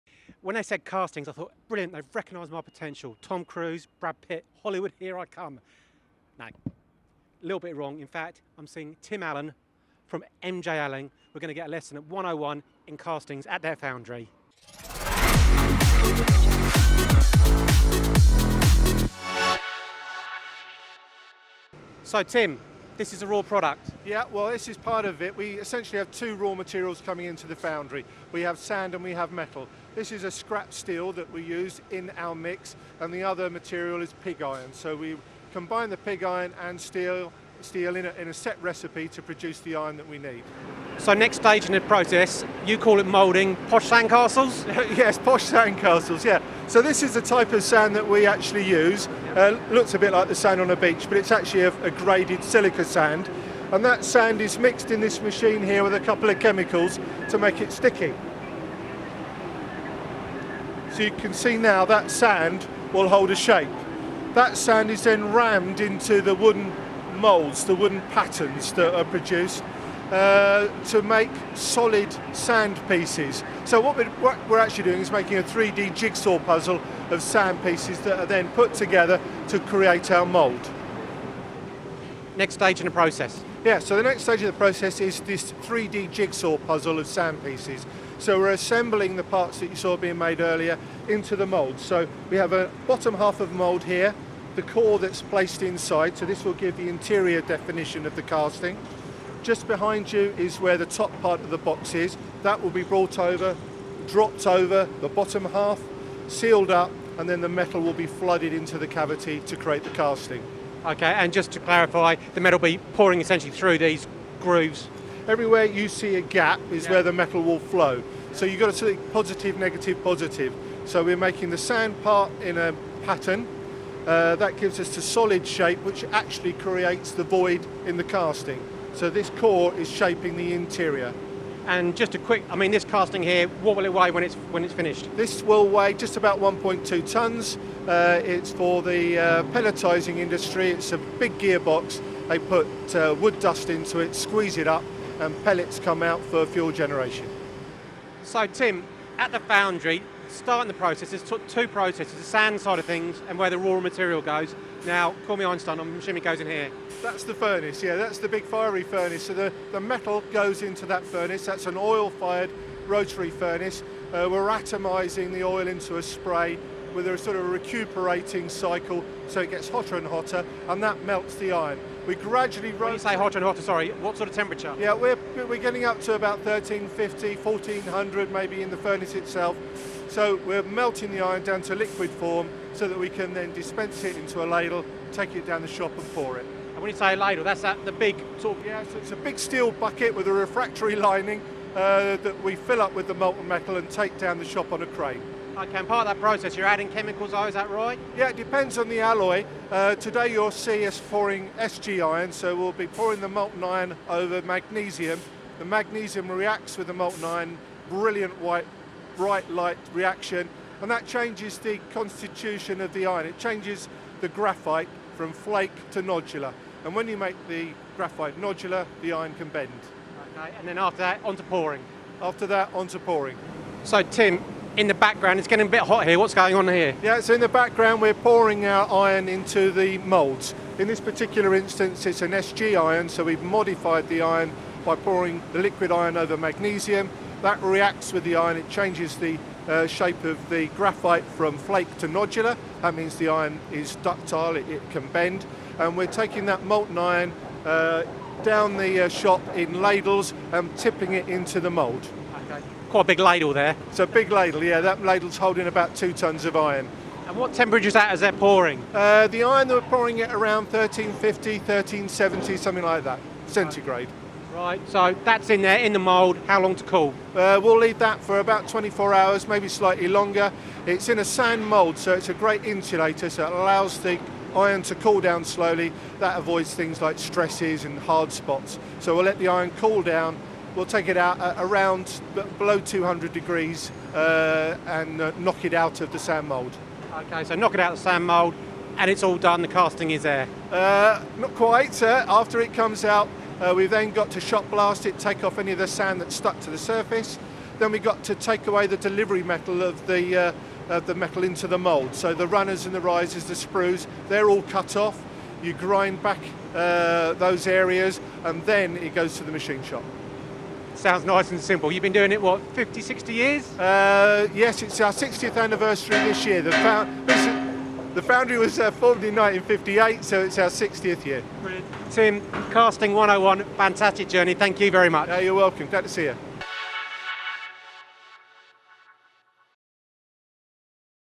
Now This Is Hot - A simple Guide to Castings
We recently dropped into MJ Allen, based in Ashford, Kent. As Tim says, they are a machine shop that makes their own castings.

In this video we do a quick walk through of how they make a casting from pig iron, smelting to pouring! And yes, it was extremely hot!

FOR PRECISION ENGINEERS based in the UK, looking to promote their company/capacity.